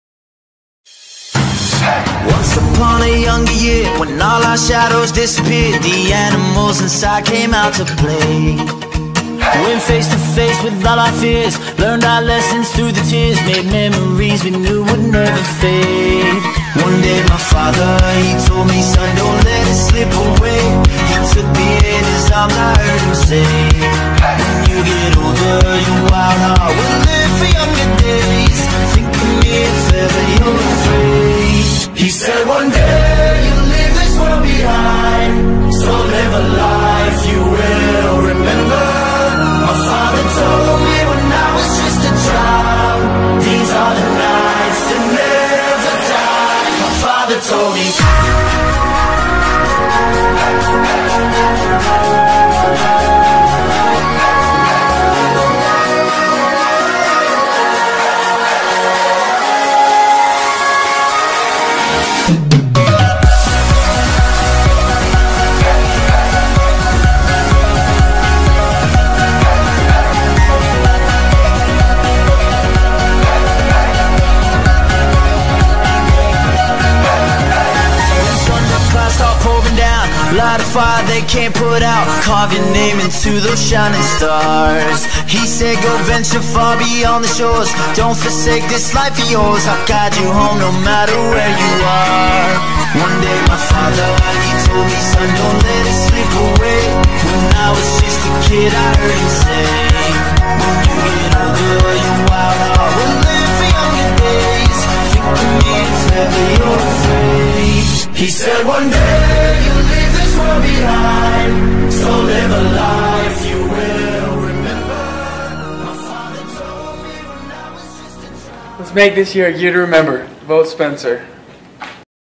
Spencer Sorensen Corner Canyon SBO 2015
Spencer Sorensen for Corner Canyon High School Student Body Officer.  A YEAR TO REMEMBER - VOTE Spencer Sorensen SS CCHS SBO